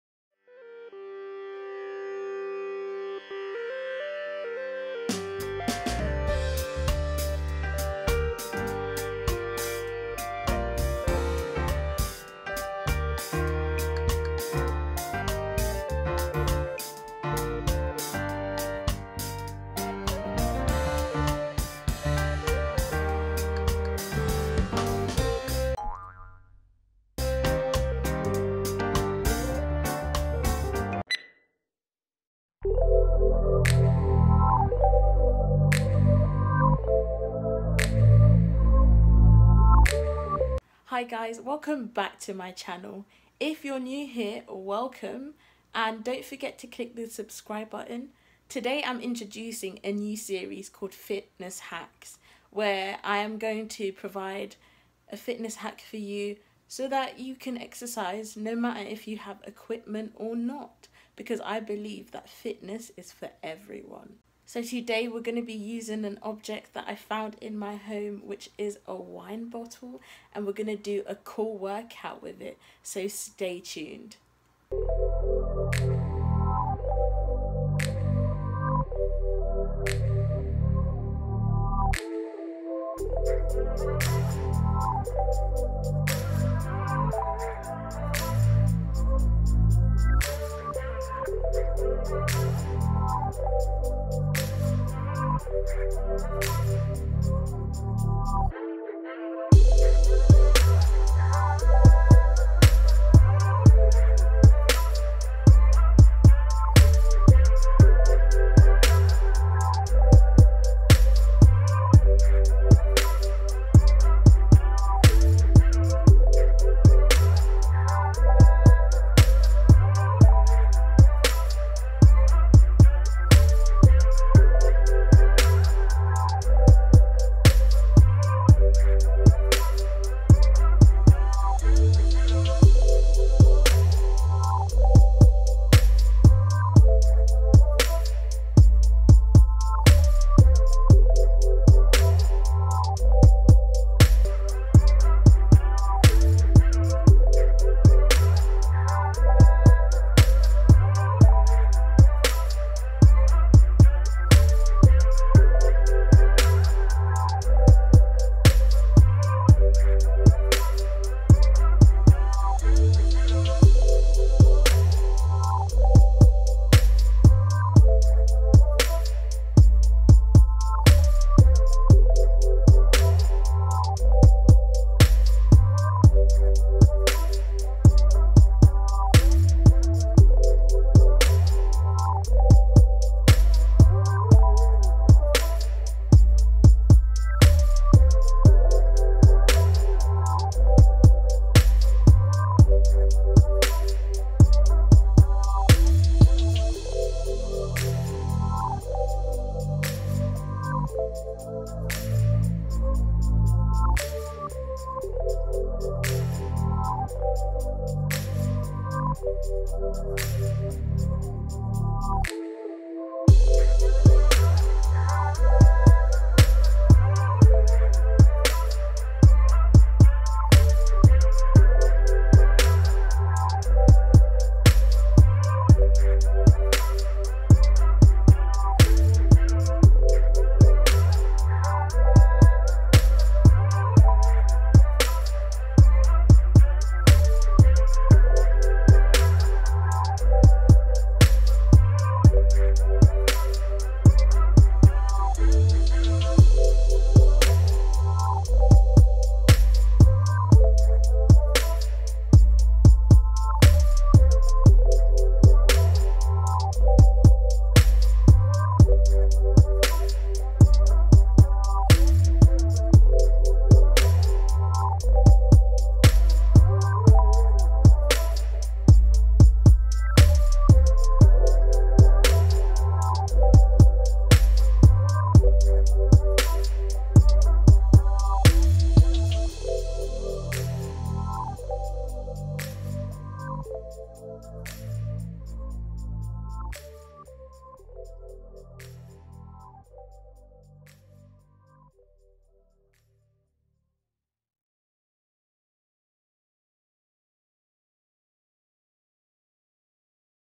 Fitness Hack (Ep 1) - Workout with a Wine Bottle?!
Welcome to my Fitness Hack Series where I will find creative ways to use objects in my house and surrounds to provide an effective workout, just as if I was using proper equipment, to encourage everyone to workout. I truly believe that FITNESS IS FOR EVERYONE!

Perform each exercise for 40 seconds and repeat the whole thing three times!

follow my fitness page @losetrashwithash for more content :)